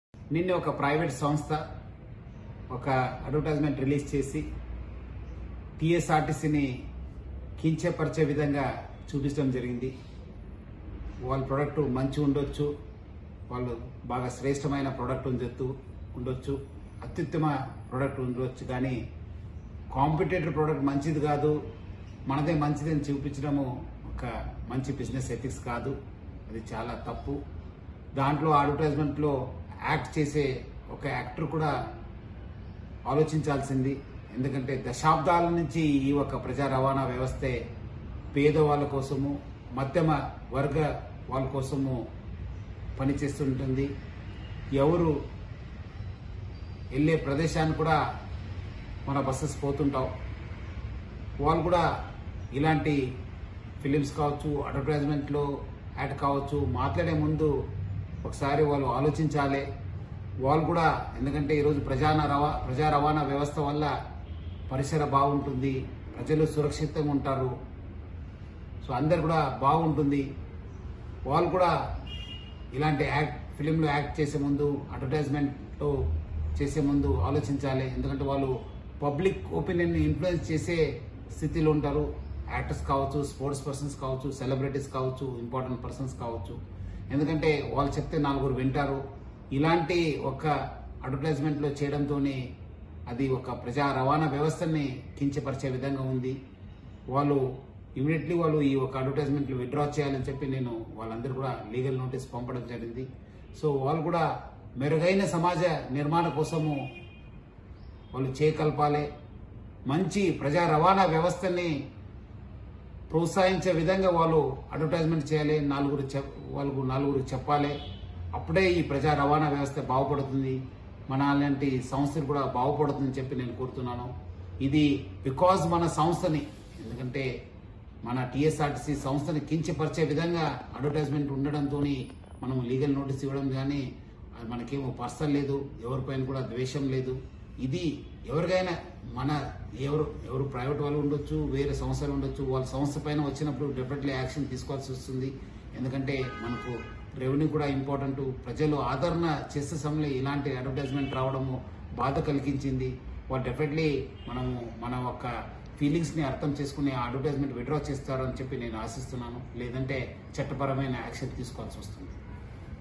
అల్లు అర్జున్ కి సెకండ్ డోస్ ఇచ్చేసిన సజ్జనార్ | Sajjanar Fires On AlluArjun Making Rapido Ad | SME
Watch : అల్లు అర్జున్ కి సెకండ్ డోస్ ఇచ్చేసిన సజ్జనార్ | Sajjanar Fires On AlluArjun Making Rapido Ad | SME